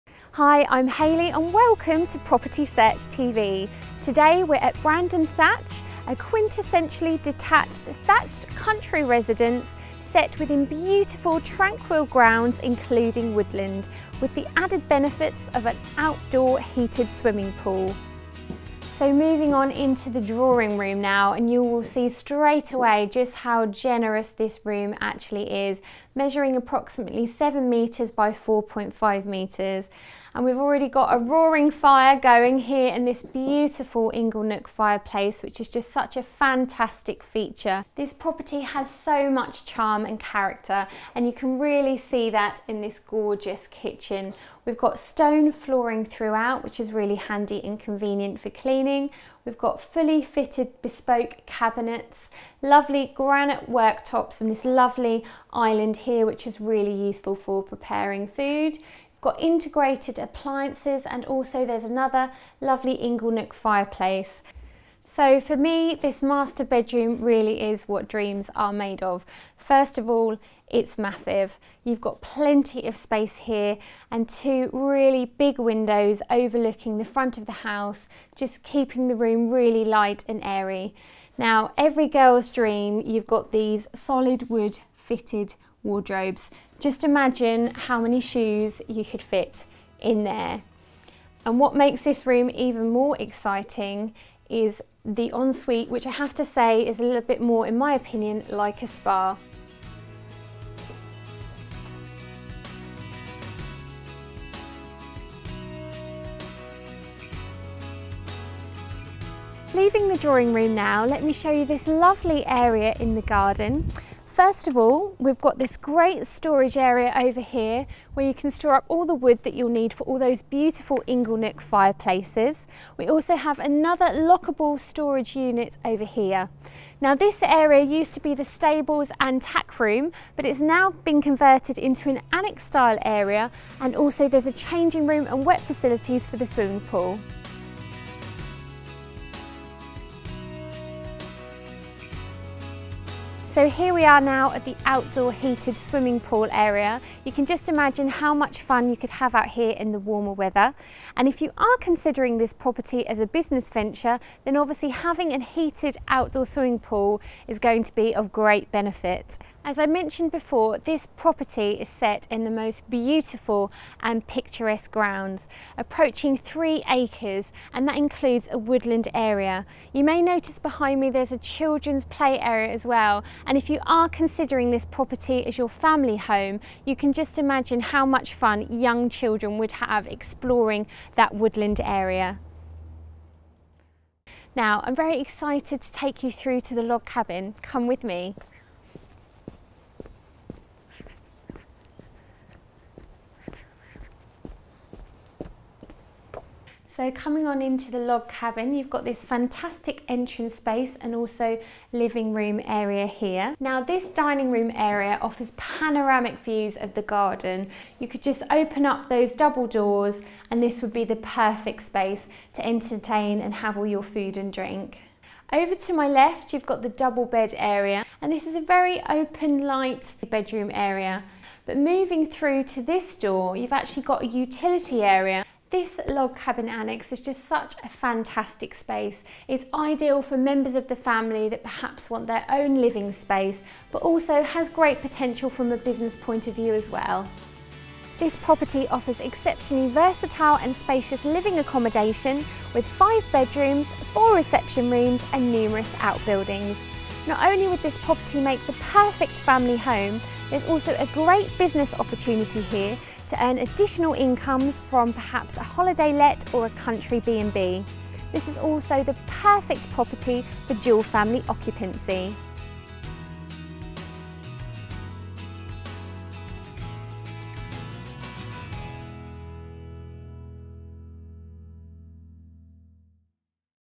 5 bedroom property for sale in Bagnum, Ringwood - Offers in excess of £1,300,000
Quintessential detached thatch residence with origins thought to date back to the mid 1600's. The property offers extensive accommodation and provides potential for holiday lets/country bed and breakfast/business use or dual family occupation. With a variety of versatile outbuildings there is the option to suit a purchaser's individual and varied requirements. Unusually for a thatch property, the ceiling heights are all generously high and the property is exceptionally light and airy throughout. there is potential to extend into the roof space and the roof space has been prepared and altered ready for extending with planning permission previously granted. For those buyers who aren't so keen on a thatch property, then the property is not listed and the roof could easily be replaced with slate tiles or suchlike. This delightful property is set within almost three acres, Brandon Thatch is approached via a tranquil country lane and offers seclusion in this sought after area. The property offers light and airy accommodation throughout but without the restrictions of low ceilings and retains many character features including Inglenook fireplaces, exposed timbers and floorboards, integrated appliances, outdoor heated swimming pool, electric gates and security alarm system.

Outside

The property is approached via electric gates which lead to an extensive parking area providing parking for numerous vehicles and a turning circle. The front of the property is mainly laid to shingle and lawn with flower and shrub borders. The rear of the property is predominantly laid to lawn with mature trees, flower and shrubs. There is an outdoor heated swimming pool measuring approximately 32' x 16' together with a large terraced area and workshop. The former stables and tack room are now being utilised as a lounge area with a bedroom, however this area has previously been an used as an office/games room with power points to cater for such use. This area also provides a wet room and shower. A timber log cabin provides a sitting room and dining area, double bedroom, shower/cloakroom and utility.

Situation

Burley is about 5 miles from the Avon Valley market town of Ringwood where a choice of supermarkets can be found together with a range of shopping and professional services. Burley offers a Junior School and falls within the catchment for Ringwood School both having exceptional reputations. The New Forest National Park offers opportunities for walking and riding over thousands of acres of moor, heathland and woodland. The A31 dual carriageway is within a couple of miles at Picket Post giving fast access to Bournemouth, Southampton, both having international airports.